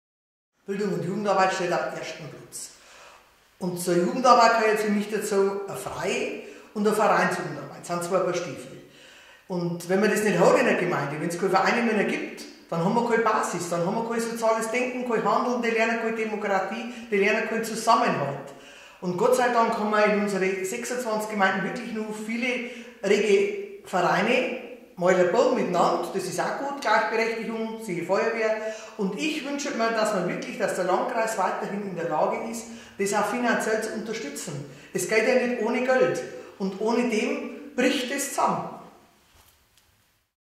Welchen Wert hat für Sie die Jugendarbeit im Landkreis Tirschenreuth?
Die Kandidaten zur Landratswahl 2014 beantworten vier Fragen, die vor allem junge Menschen im Landkreis Tirschenreuth betreffen. Diese Aktion wurde vom Kreisjugendring Tirschenreuth initiiert.
Das Jugendmedienzentrum T1 produzierte die Videos.